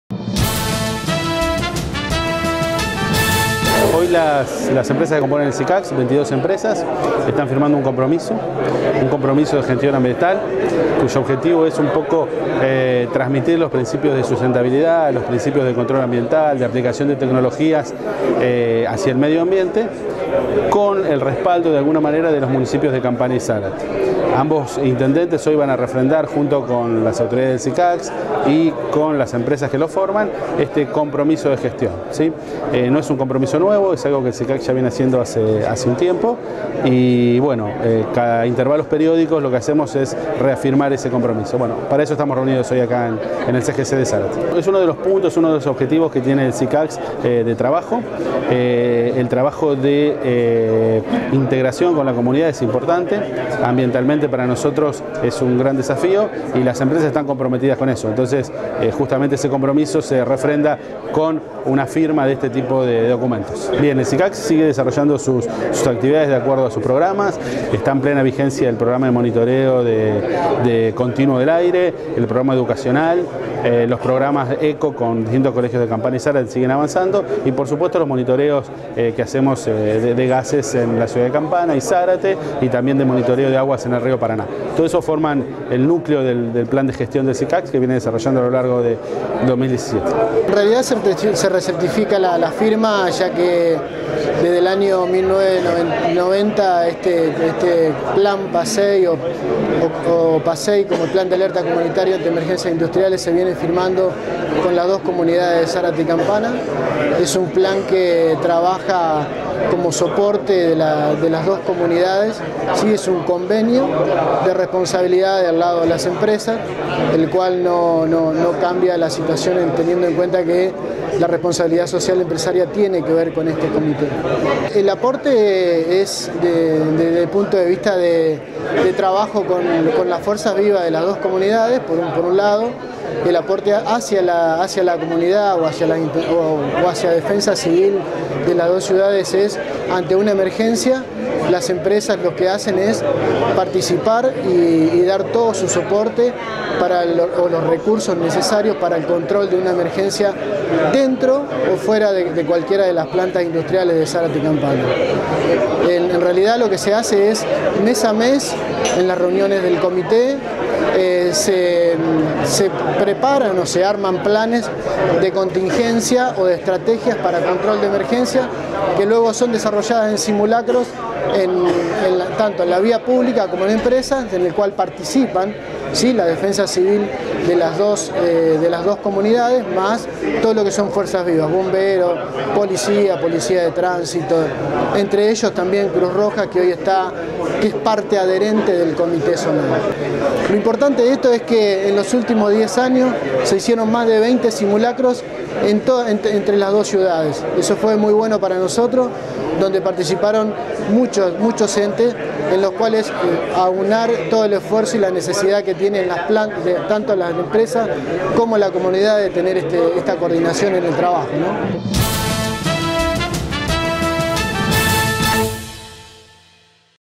Principio de gestion ambiental
Los municipios de Campana y Zárate, junto con las empresas que forman el CICACZ, adhirieron a los principios de gestión ambiental.  El objetivo es lograr la conservación del ambiente en la región.